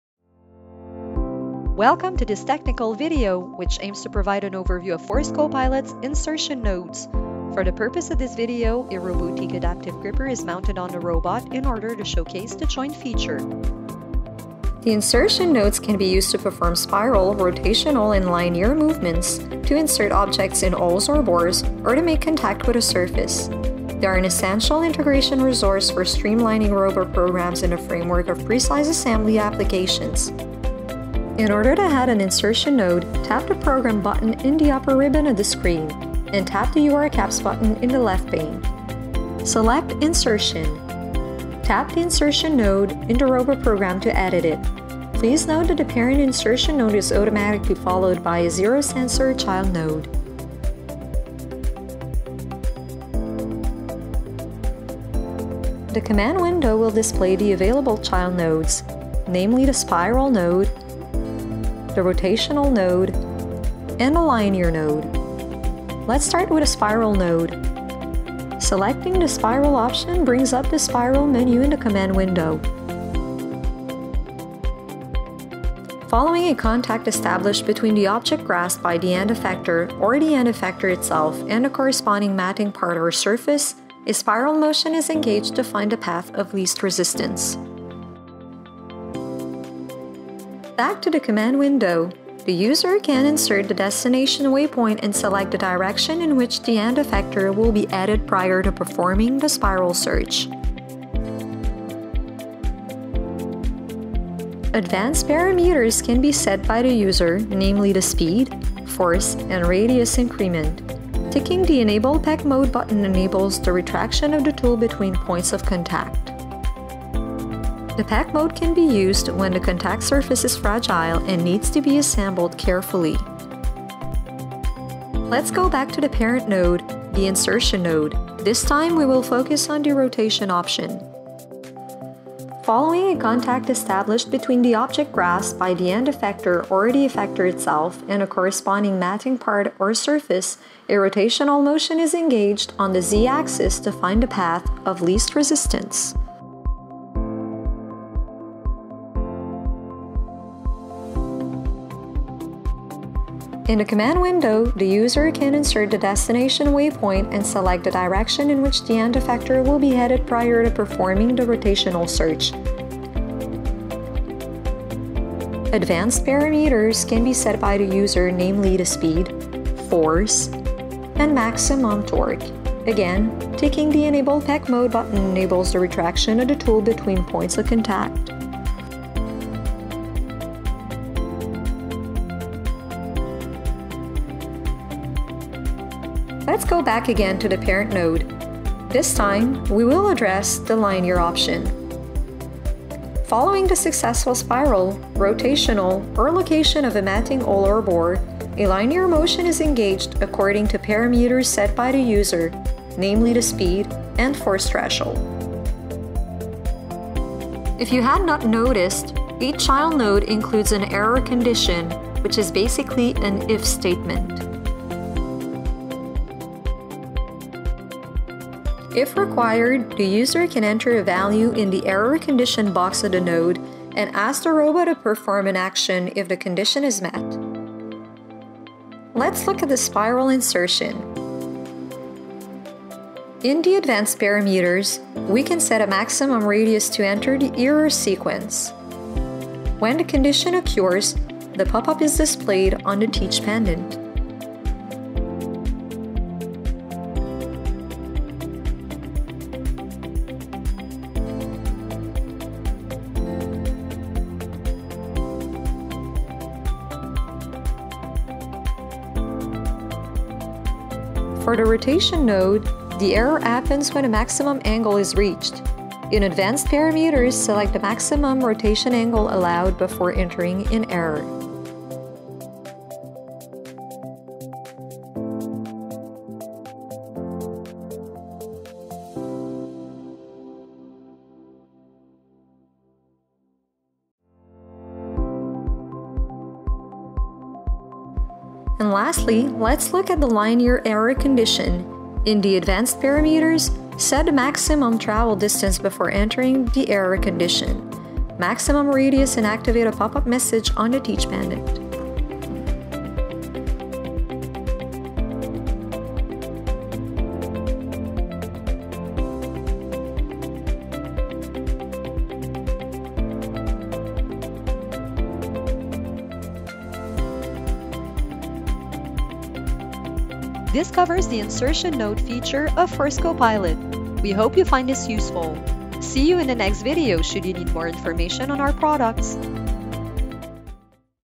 How to Perform an Insertion with a Collaborative Robot
Learn how to perform insertions with a Universal Robots by using Robotiq's Force Copilot. Work with spiral, rotational and linear search nodes to program an insertion in minutes and ensure a repeatable process. Want to learn more?